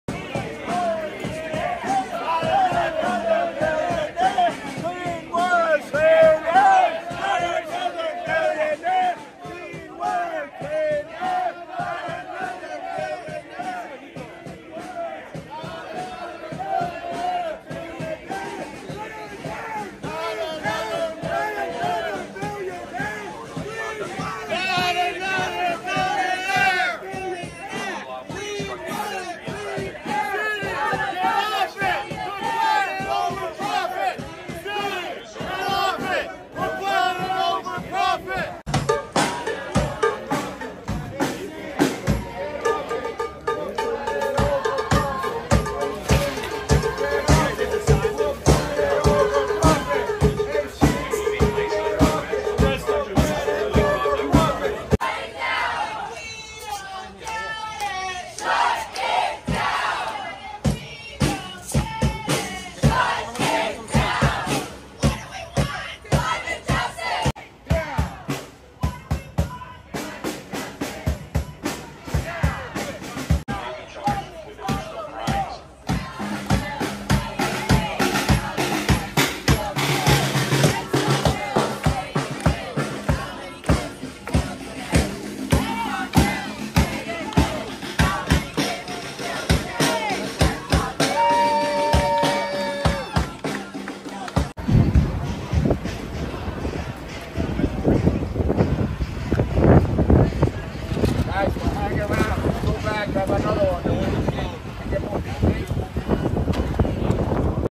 Arrests Made as Climate Protesters Block Citigroup Offices in Manhattan
At least 23 climate protesters were arrested on Monday, June 10, after blocking entrances to Citigroup offices in Lower Manhattan, local media reported.
Credit: Brendan Gutenschwager via Storyful